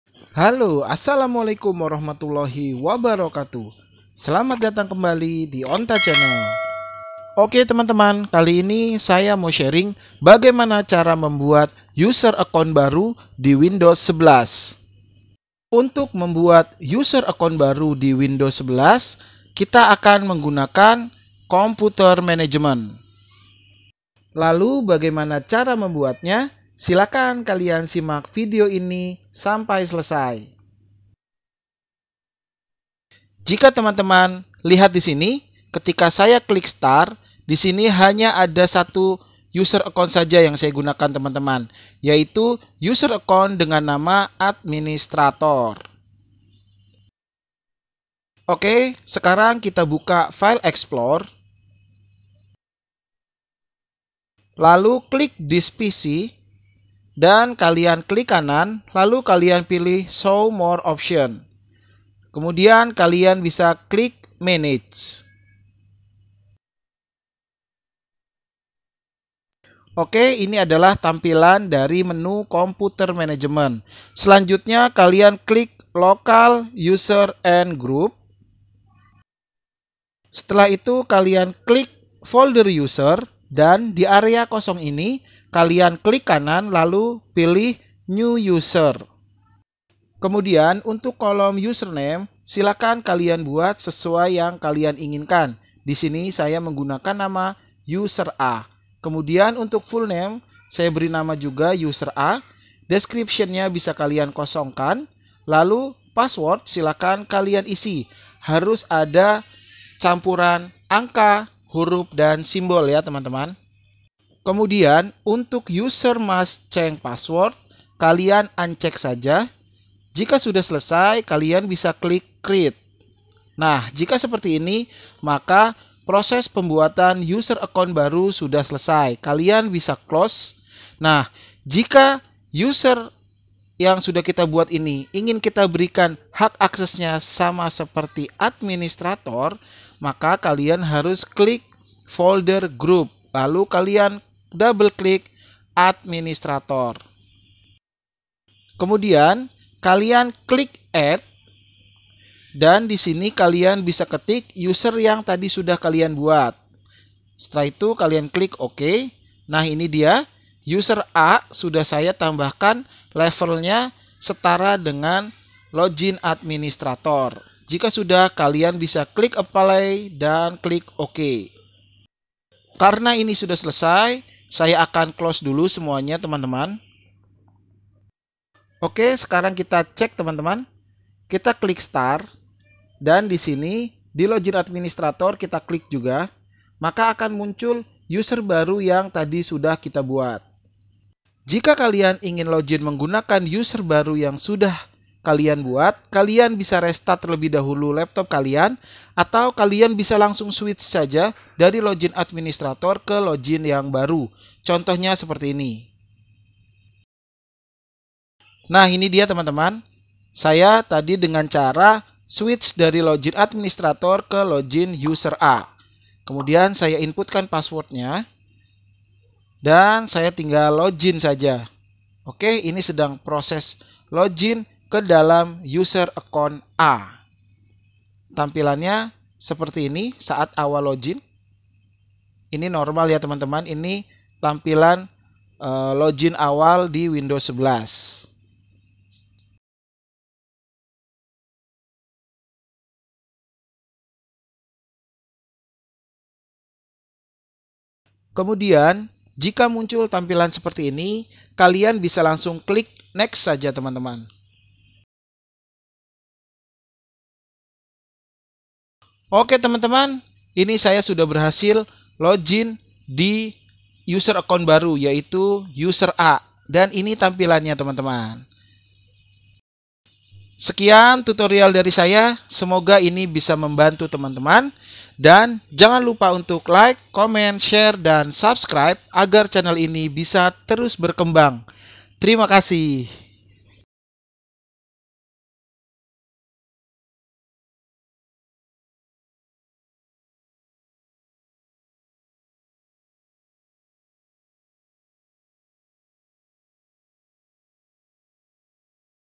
Cara Membuat User Account Baru Di Windows 11
Selamat datang di onta channel.

User account pada Windows 11 adalah akun yang dibuat oleh pengguna untuk mempersonalisasi pengalaman mereka dengan sistem operasi Windows 11. Akun ini memberikan akses ke fitur dan pengaturan yang berbeda, seperti pengaturan privasi, pengaturan aplikasi, dan berkas pribadi. Pengguna dapat membuat beberapa akun untuk berbagi komputer dengan orang lain.

Channel ini tentang kegiatan saya dalam belajar IT (Informasi dan teknologi).

Semoga channel ini bisa bermanfaat untuk teman teman dan juga saya.